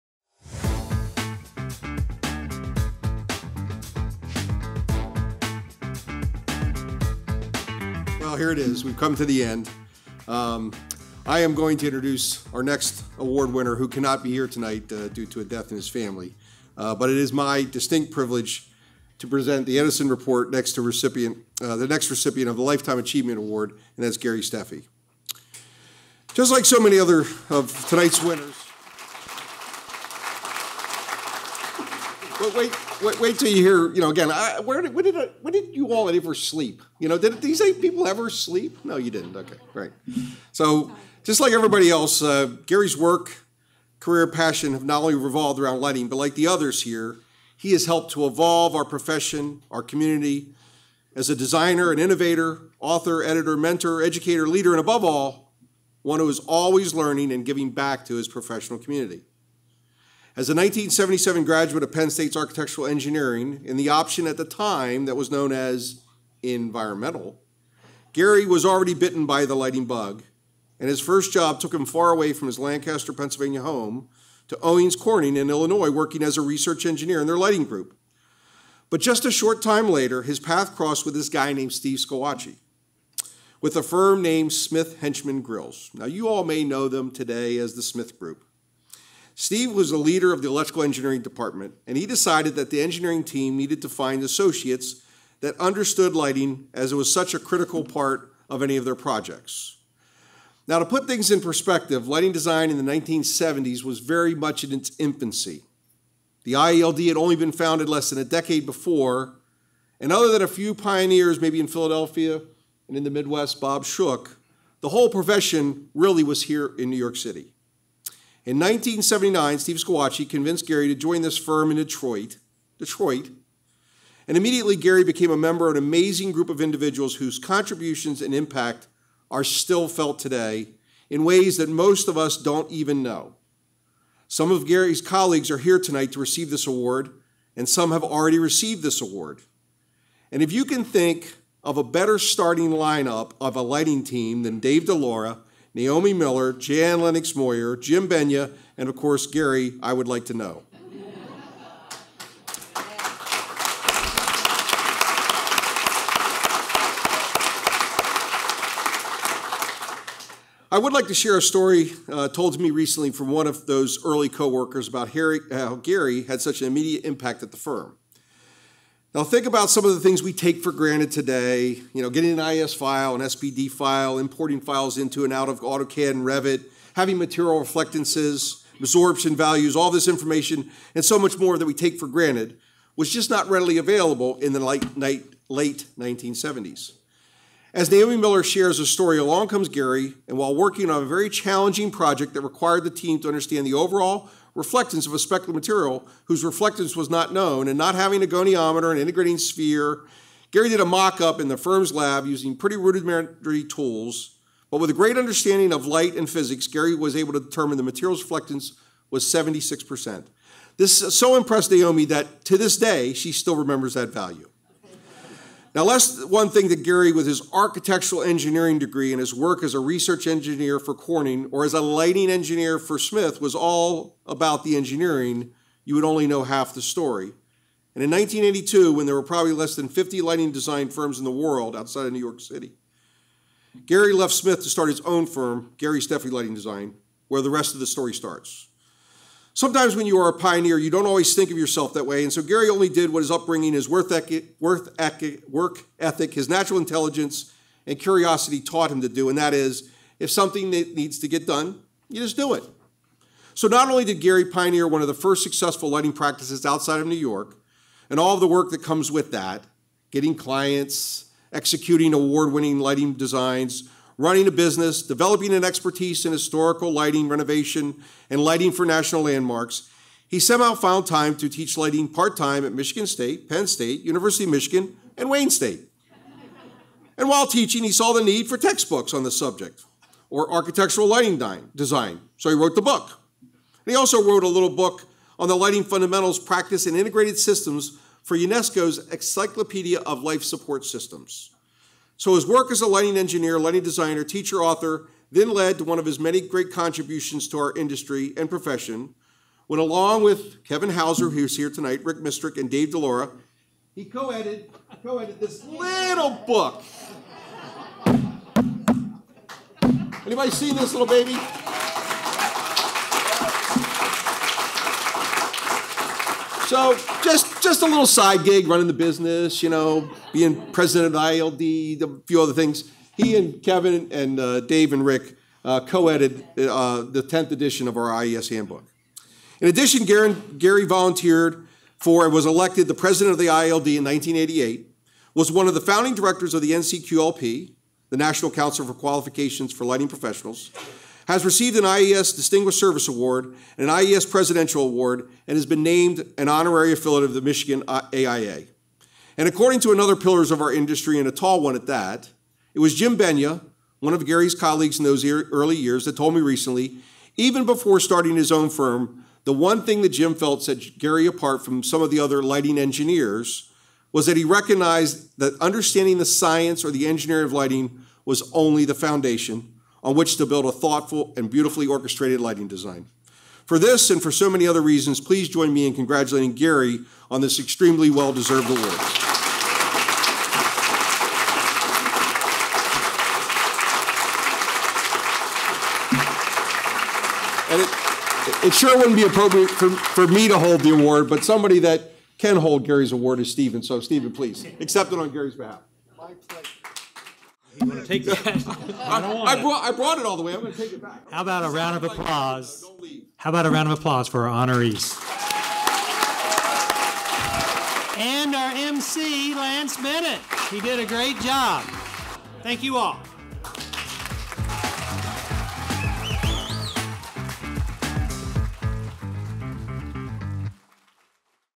EdisonReport 9th Annual Lifetime Achievement Awards: Gary Steffy
Lance Bennet celebrates Gary Steffy at the 9th annual Lifetime Achievement Awards held at Parsons School of Design.

“Gary’s technical ability is renowned by his colleagues,” Lance remarked, recounting an anecdote of Gary spending hours on a mockup to accurately calculate a surface’s reflectance value. “But, if you thought Gary’s work was all about engineering, you would only know half the story.” Gary has succeeded at the formidable task of fusing technical knowledge with artistic creativity throughout his illustrious career.